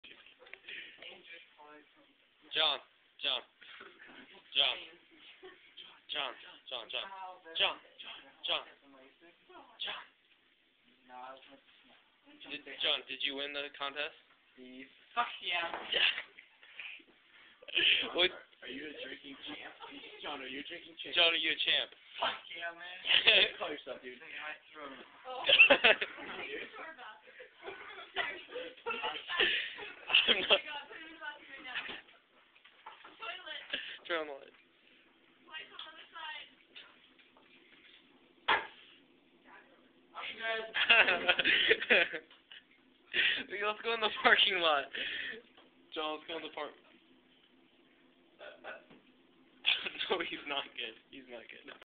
drunk SWEDY, aka hospital boy!
won a drinking contest, later went to the hospital